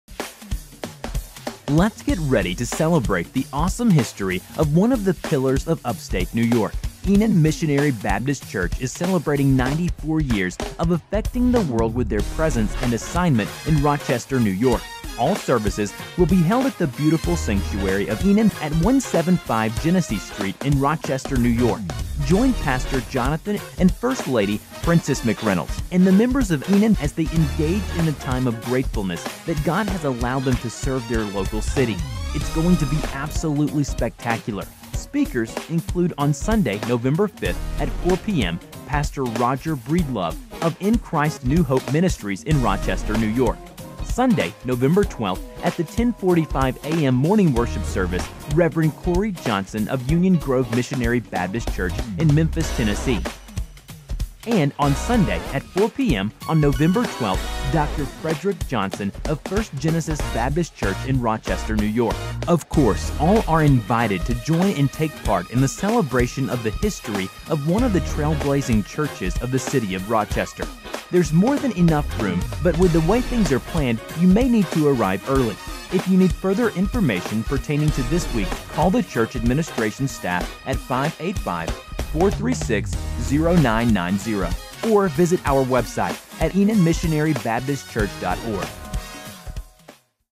Celebrating 94 Years!!
Come Celebrate AMBC'S 94th Church Anniversary!! Sunday, November 5th @4pm , Saturday, November 11th @6pm (Concert) and Sunday, November 12th @ 10:45am & 4pm